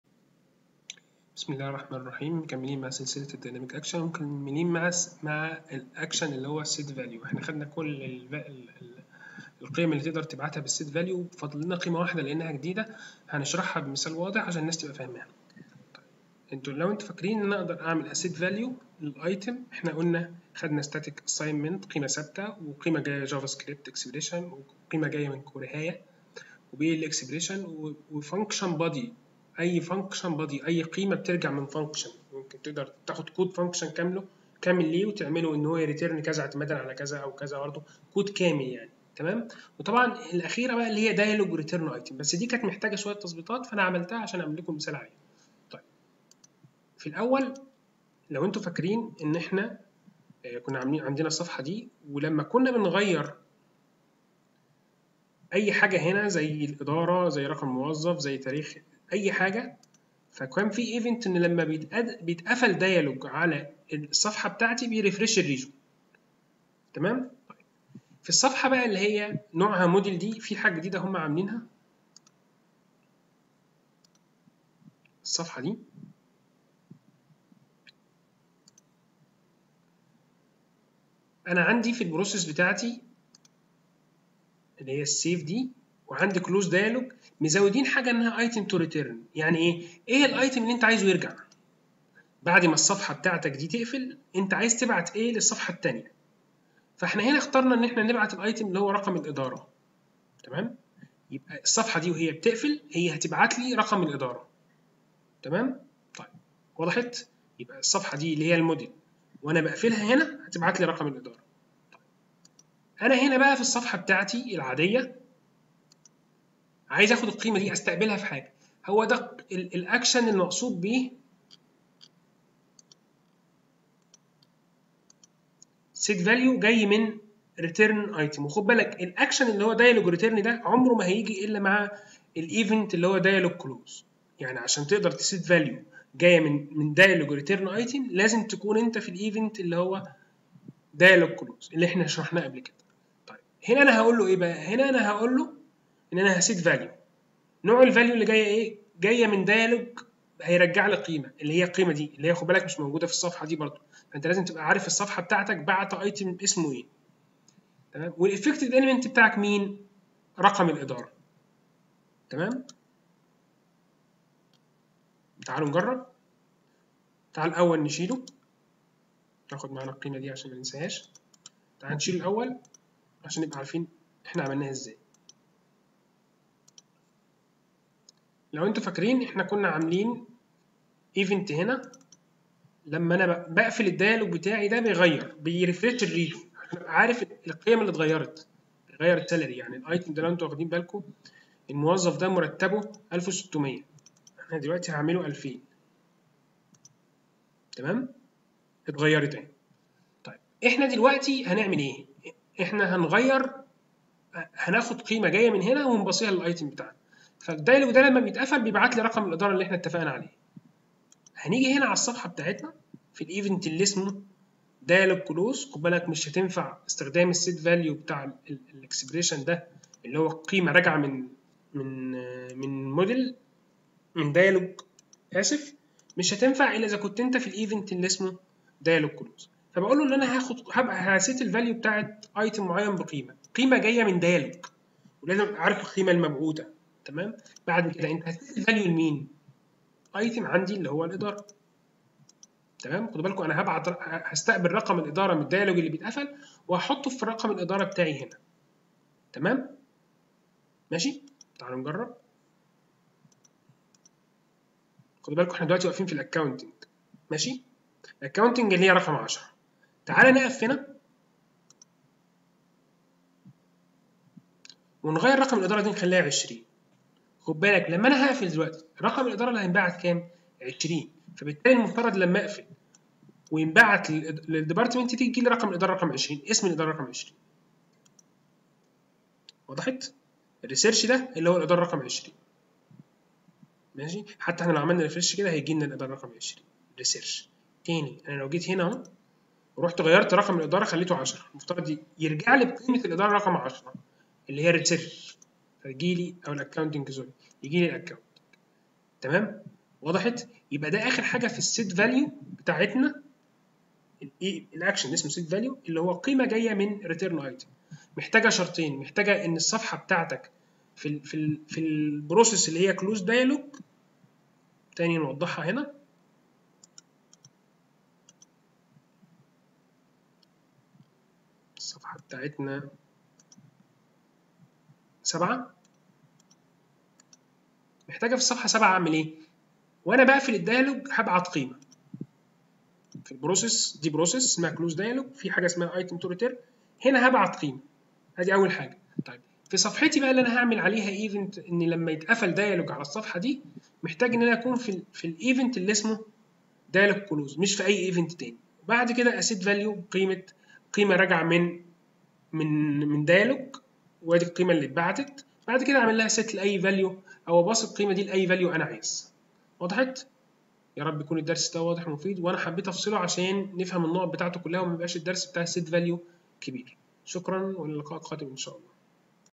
Oracle Apex 19.1 : Dynamic Action Part 25 ( with english Subtitle )
Oracle Apex 19.1 : Dynamic Action
Event:  Dialog Closed
Action : set value
Set Type:
                Dialog Return Item